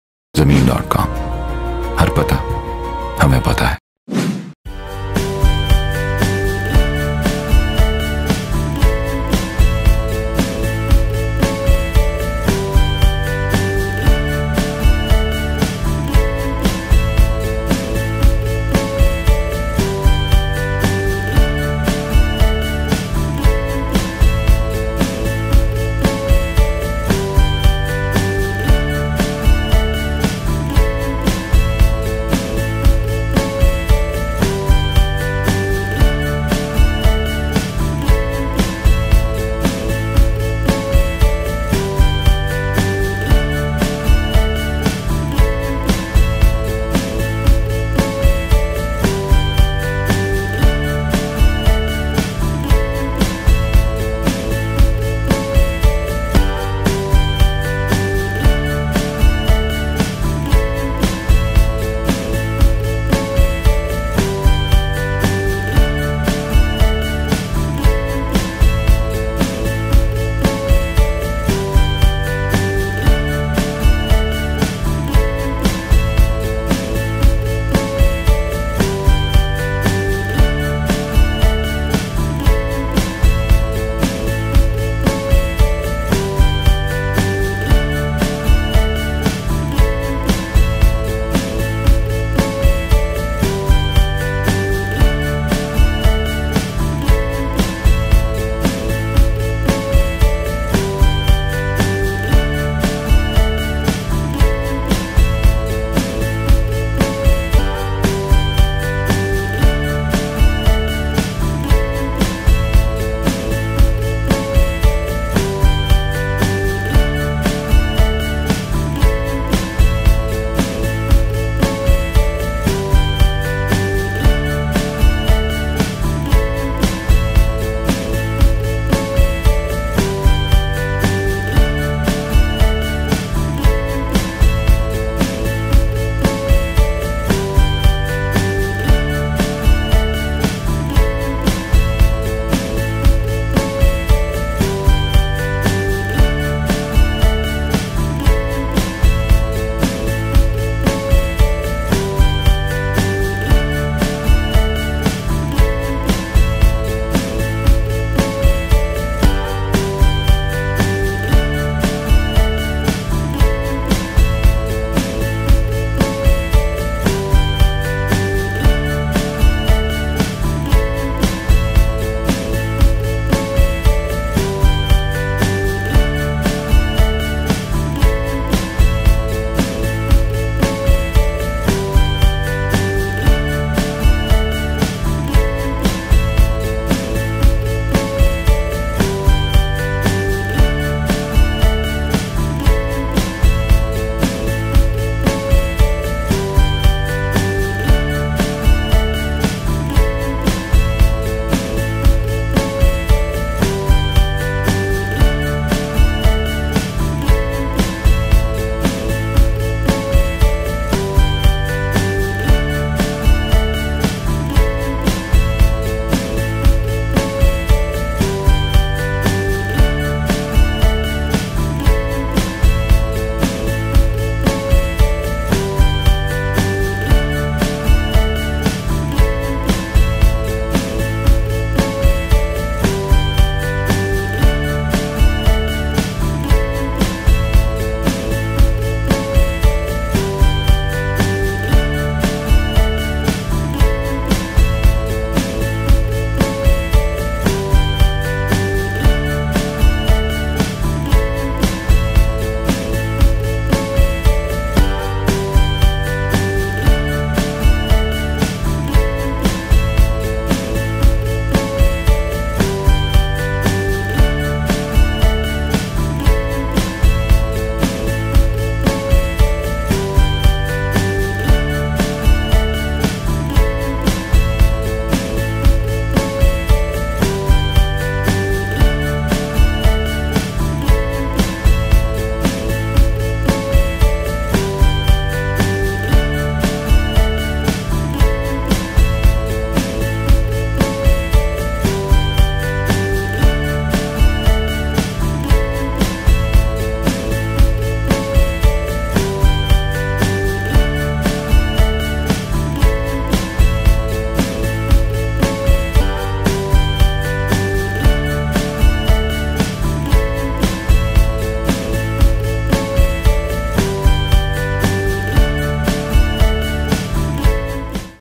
1 KANAL HOUSE FOR SALE IN GHOURI BLOCK SECTOR B BAHRIA TOWN LAHORE
If you wish to purchase a fine piece of property in Lahore, check this one. And along with its reasonable this property option is definitely a keeper.

The perfect 1 Kanal House is available for sale now.

This property is definitely not a miss! So, buy it before it gets off of your radar.

Take it from us, a House like this doesn't come easy. So you should grab the offer while you still can, before someone else does.

The Bahria Town is a deal you must take a look at.